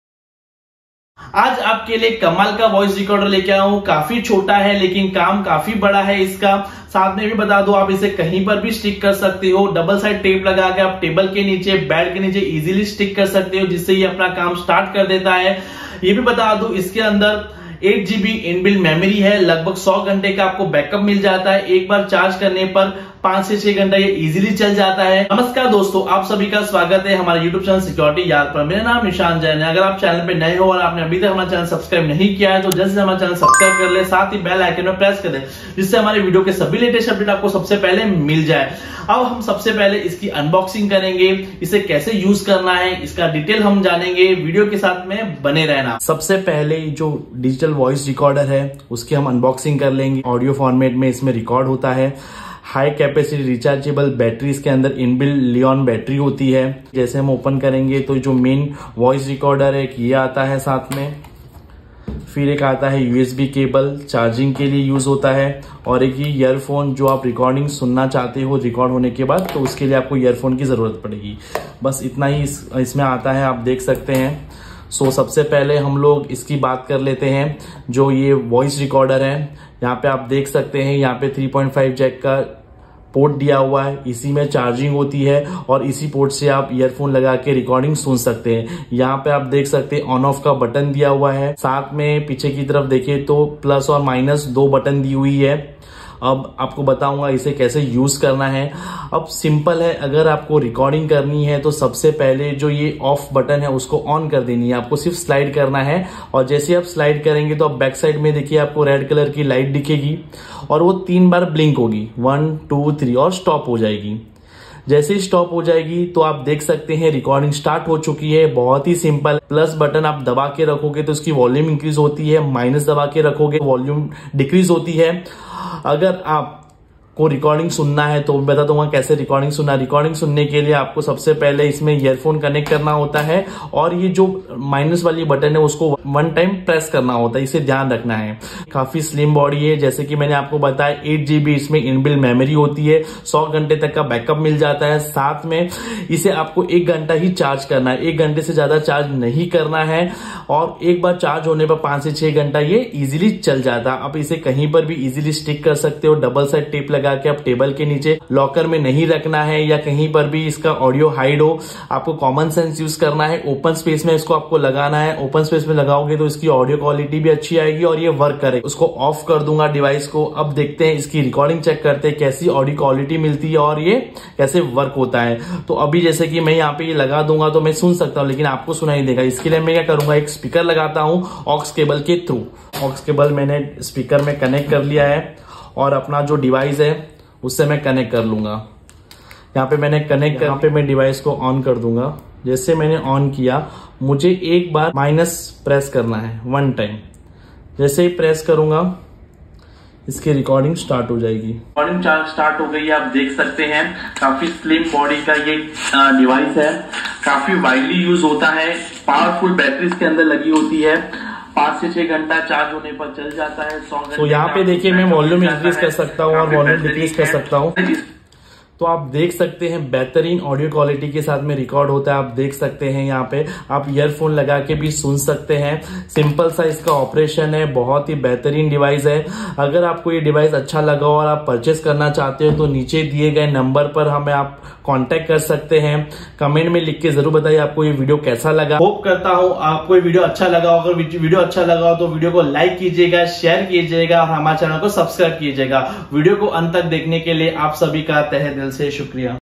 Spy Voice Recorder | Hidden Audio Recorder | Unboxing & Review
Hello Friends,
Is Video me humne Spy Voice Recorder ke baare me bataya hain. Iske sabhi features ko detail me bataya hain . Hidden audio recorder ki unboxing & complete review kiya hain.

Queries Solved:
1. Hidden Audio Recording Device
2. How Work Spy Voice Recording device
3. Voice Recording Device Unboxing